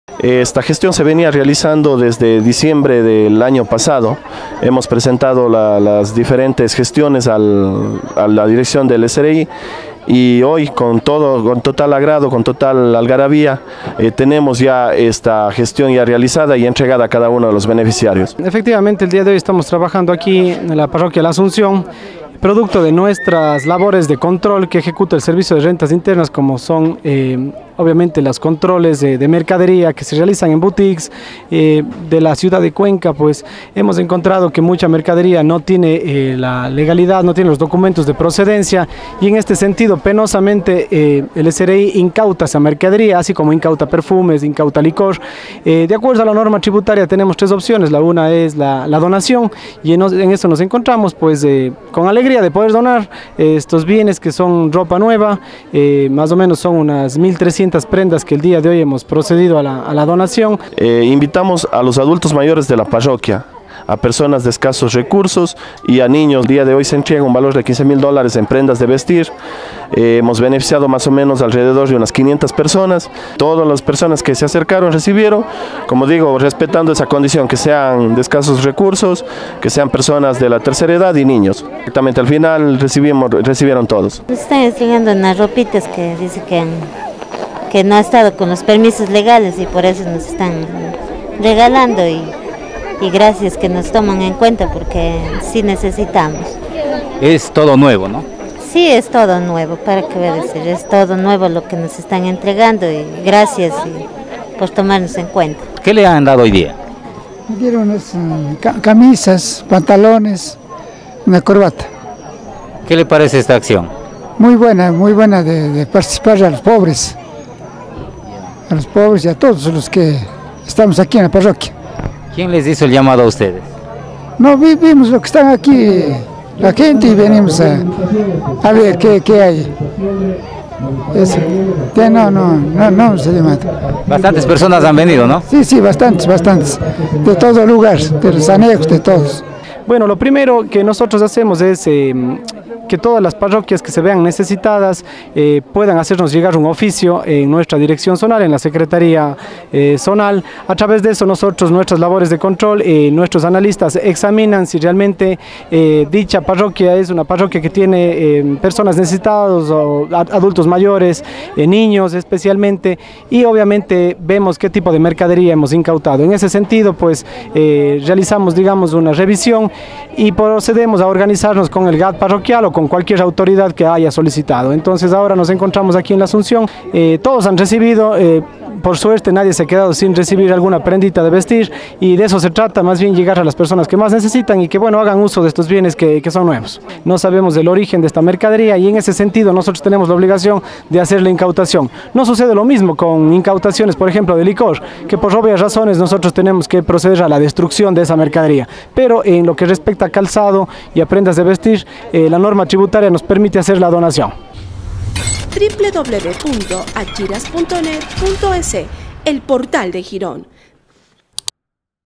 SRI dona 1300 prendas de vestir a población de La Asunción
Producto de decomisos a comercios que tienen mercancías sin los respaldos de facturas en la ciudad de Cuenca, el Servicio de Rentas Internas (SRI) cuenta con mercancías como ropa, perfumes y licores. De acuerdo a la ley, existen formas de dar destino a los decomisados, las ropas y calzados son donados, en tanto que licores destruidos. El SRI, de acuerdo a las gestiones de las autoridades de los poblados dona la ropa y calzado. La semana pasada la oportunidad fue a los habitantes de la parroquia La Asunción, donde unas 500 personas se beneficiaron de 1300 prendas valoradas en 15 mil dólares.